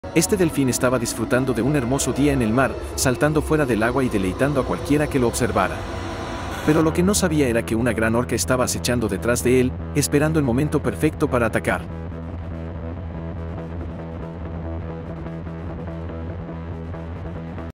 Delfin es golpeado por orca | espectaculo natural
Este delfín estaba disfrutando de un hermoso día en el mar, saltando fuera del agua y deleitando a cualquiera que lo observara. Pero lo que no sabía era que una gran orca estaba acechando detrás de él, esperando el momento perfecto para atacar